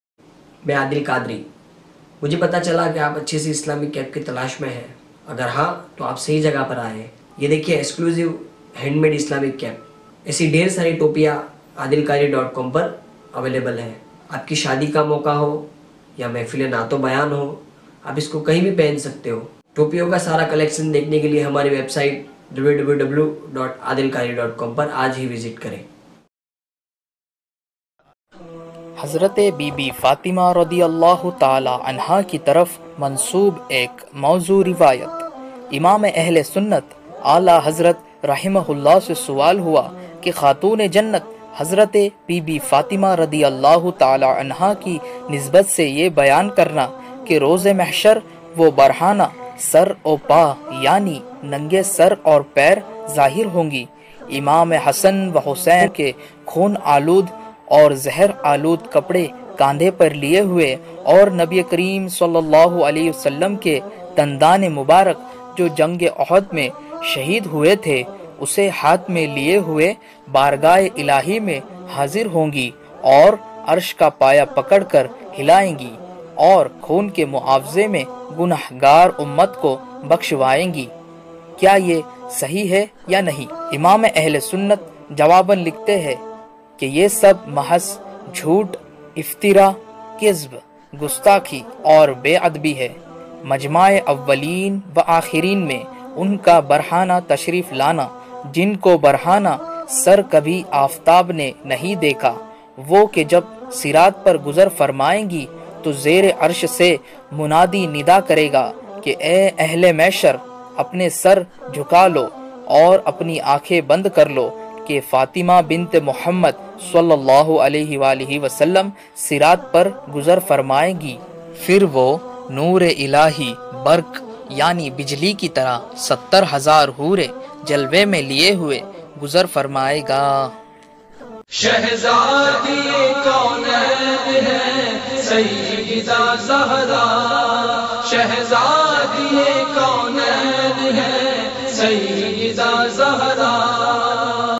Hazrat Fatima Ka Jhoota Waqiya | Alahazrat Ka Fatwa | PURE SUNNI INTERNATIONAL
Click To Order Adil Qadri Cap

Hazrate Fatima Ki Taraf Mansoob Ek Mauzu Riwayat

Imam -e- Ahle Sunnat, Aala Hazrat Rahimahullah Se Sawal Hua Ke Khatoon -e- Jannat, Hazrat Fatima Radiallaho Ta'ala Anha Ki Nisbat Se Ye Bayaan Karna Ke Roze Mehshar Wo Barhana Sar -o- Paa (Yaani Nange Sar Aur Pair) Zaahir Hongi, Imam Hasan Wa Husain Ke Khoon Aalud Aur Zahar Aalud Kapde Kandhe Par Liye Huye Aur Nabiye Kareem ﷺ Ke Dandan Mubarak Jo Jung -e- Uhud Mein Shaheed Huye The Use Haath Mein Liye Huye Bargaah -e- Ilahi Mein Haazir Hongi Aur Arsh Ka Paaya Pakad Kar Hilayengi Aur Khoon Ke Muawze Mein Gunahgar Ummat Ko Bakhshwayengi, Ye Sahih Hai Ya Nahin?

Imam -e- Ahle Sunnat Jawaban Likhte Hain Ke Ye Sab Mahaz Jhoot, Iftira, Kizb, Gustakhi Aur Be Adabi Hai, Majma -e- Awwaleen Wa Aakhireen Mein Unka Barhana Tashreef Laana Jinko Barhana Sar Kabhi Aaftab Ne Nahin Dekha, Wo Ke Jab Siraat Par Se Guzar Farmayengi To Zere Arsh Se Munaadi Nida Karega Ke Aye Ahle Mehshar! Apne Sar Jhuka Lo Aur Apni Aankhein Band Kar Lo Ke Fatima Binte Muhammad ﷺ Siraat Par Guzar Farmayengi Phir Wo Noor -e- Ilahi Burq Ki Tarah Sattar Hazaar Hoorein Jalwe Mein Liye Huye Guzar Farmayega

(ملخصاً: احکامِ شریعت، ص160)

Abde Mustafa
Voice Over : Syed Asad Sahab